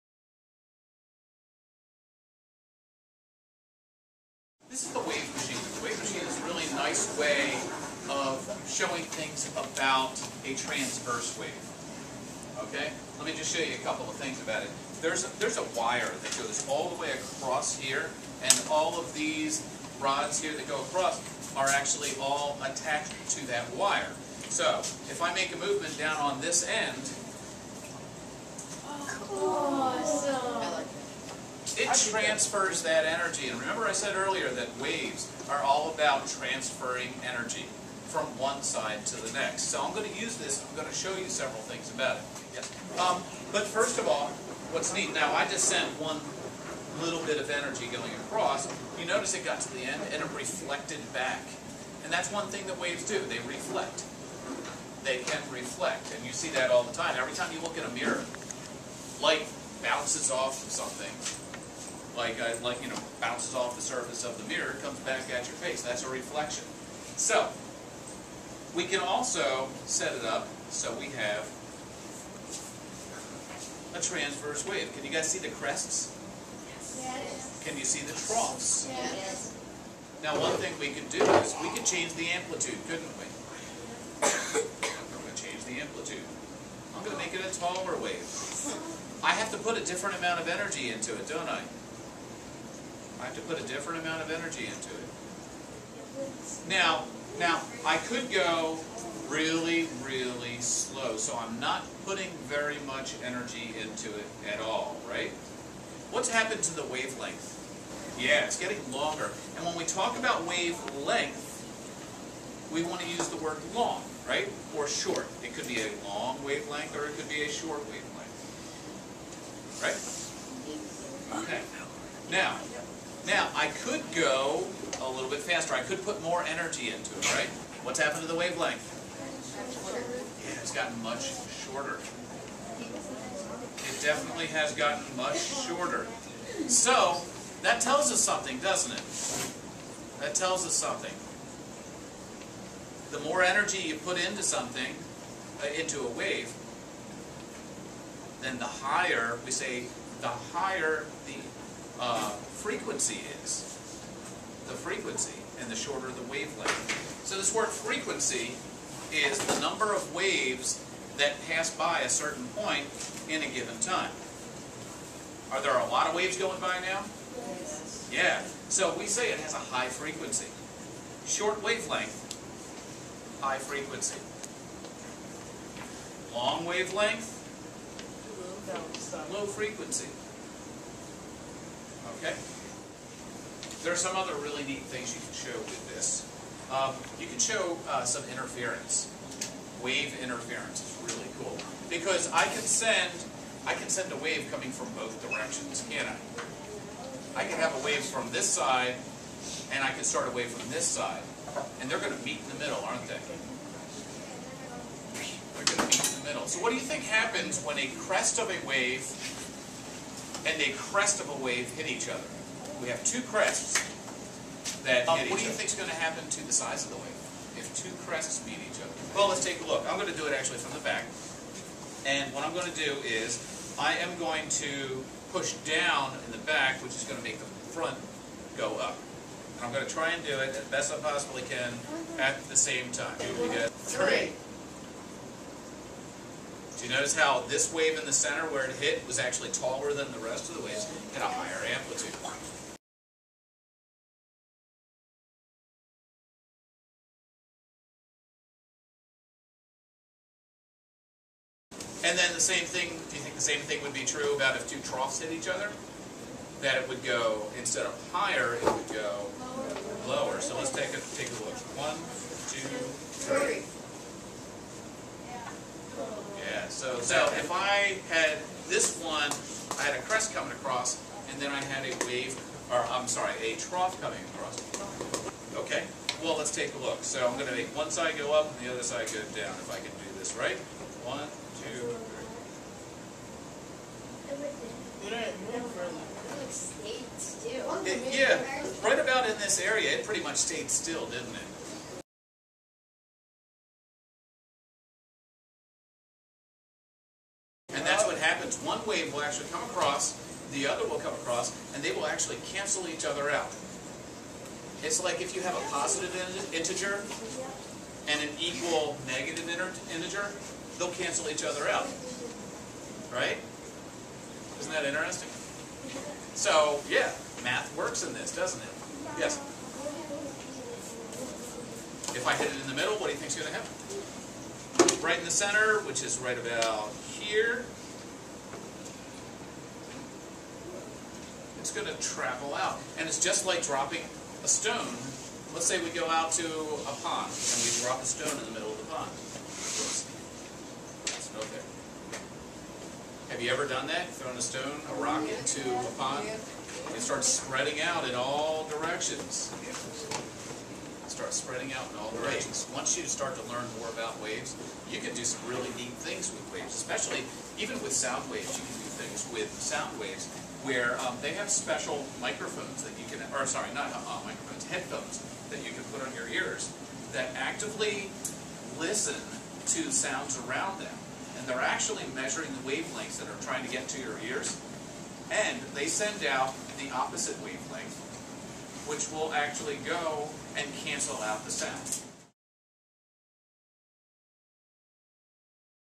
Wave Machine Demonstration
This is a demonstration for my sixth grade science class about the various properties of transverse waves.